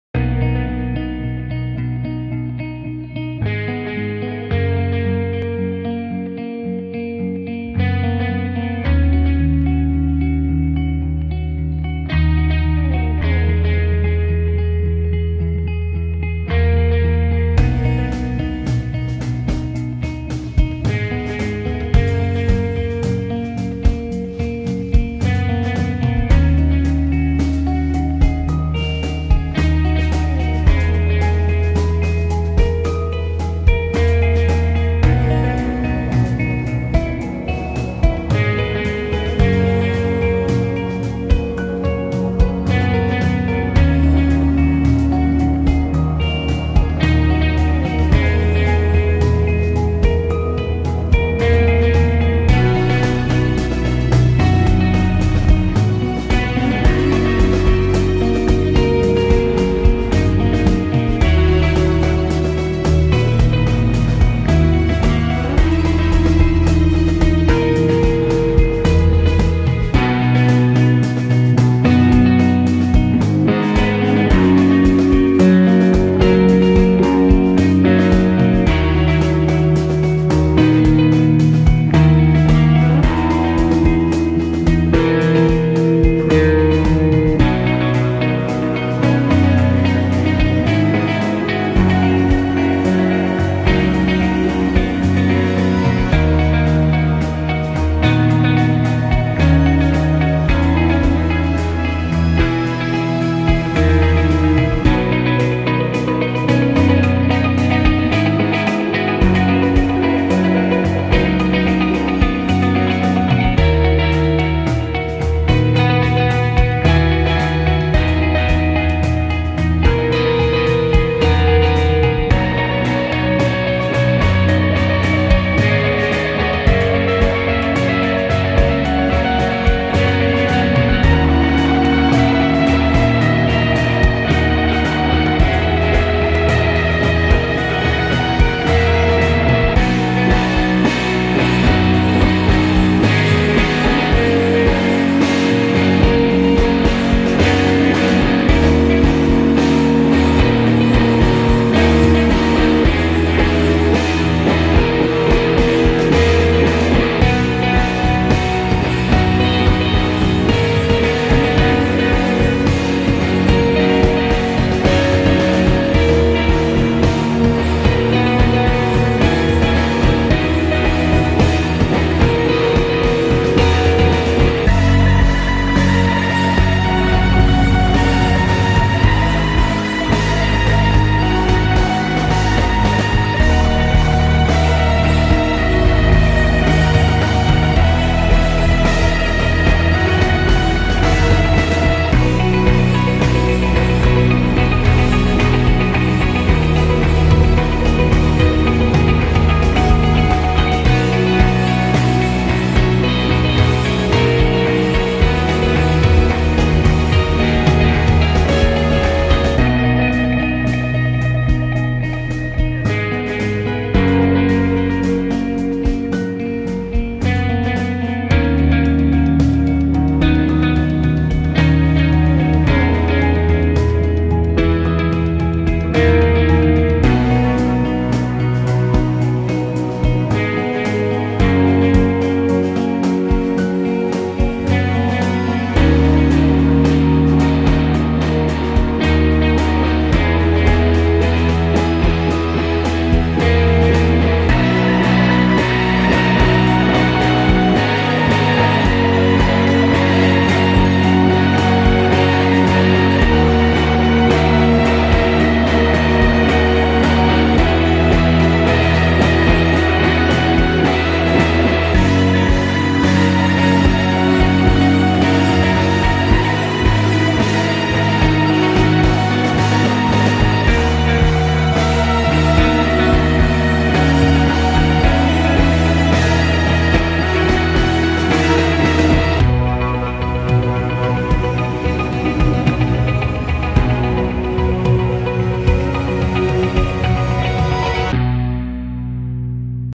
Song For My Friend Jason Zelda
I'm dumb and accidentally deleted the Song and Video I made for Jason. So luckily I didn't delete it so I uploaded it again. 🤭